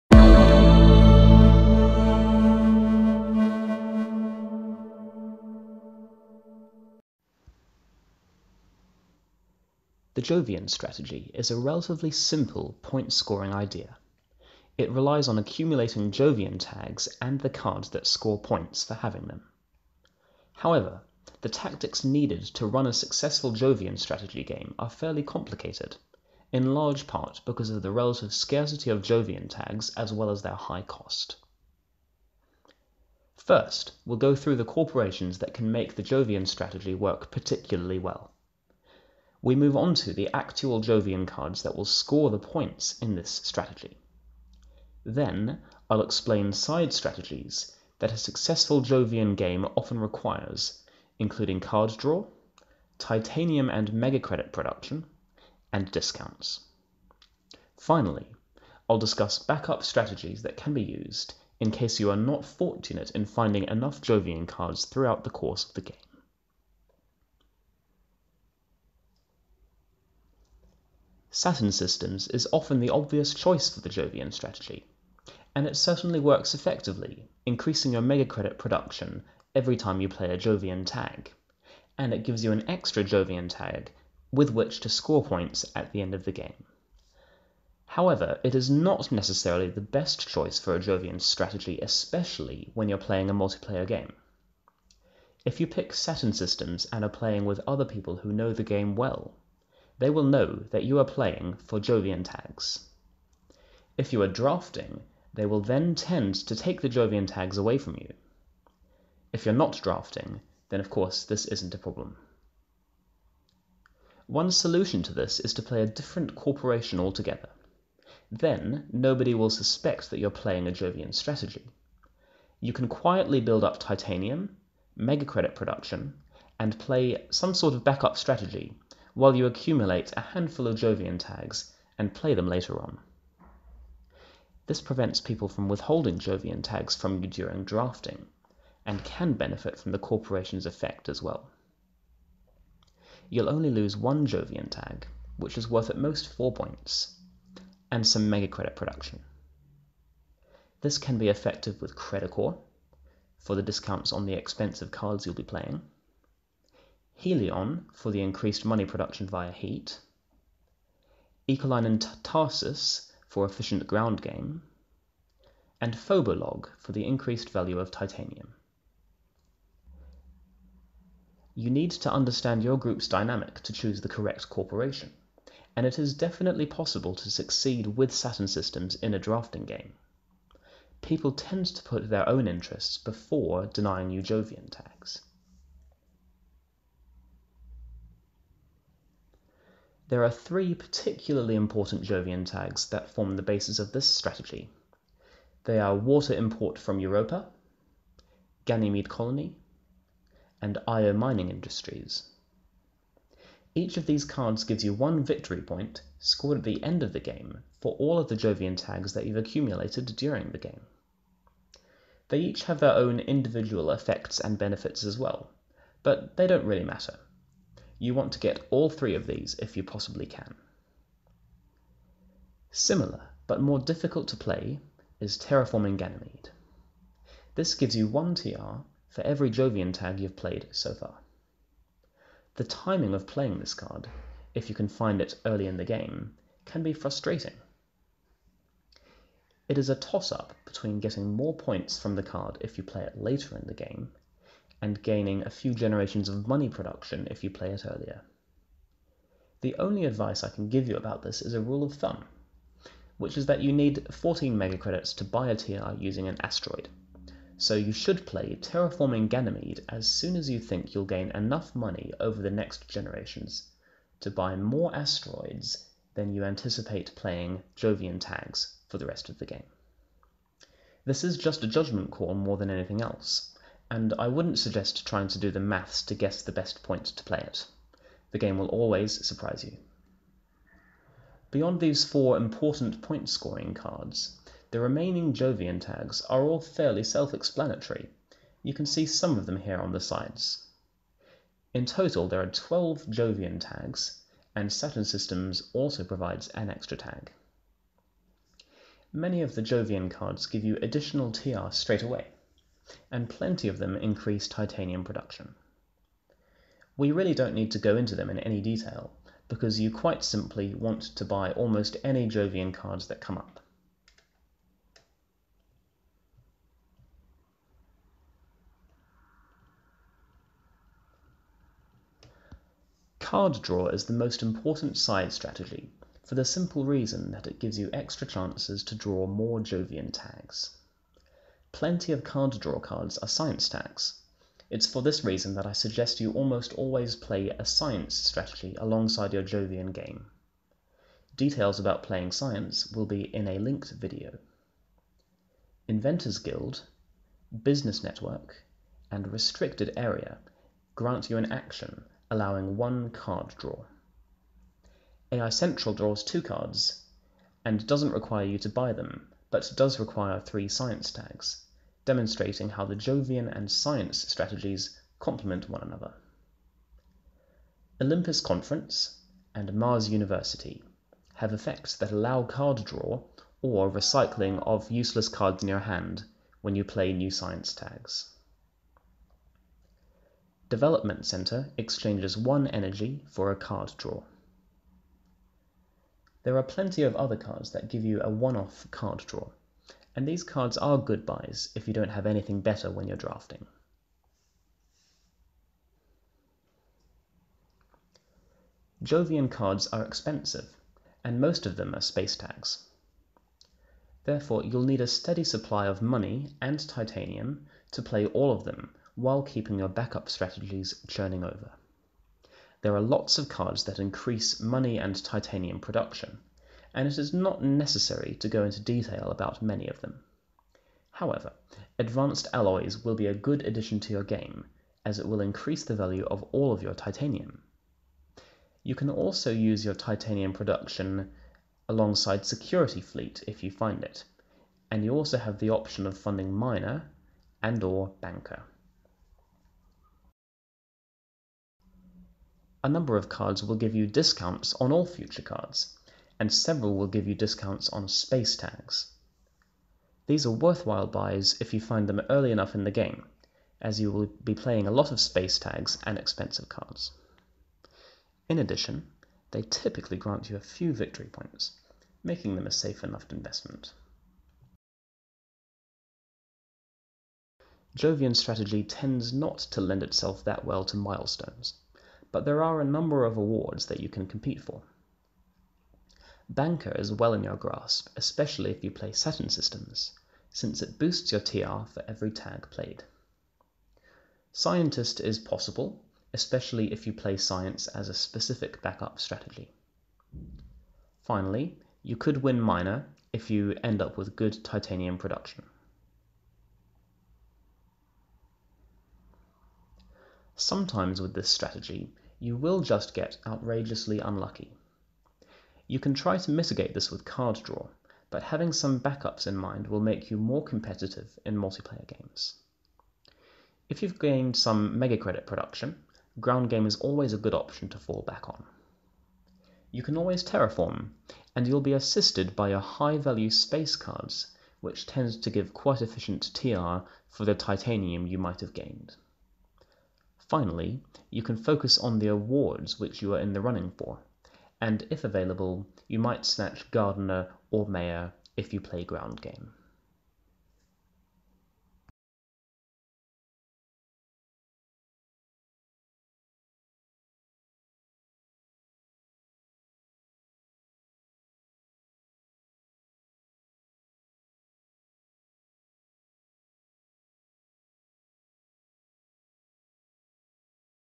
[Terraforming Mars] Strategy #3 - Jovian
Explore the strategy of Terraforming Mars! This video teaches you how to turn a point profit from your friendly local Jupiter in the Jovian strategy.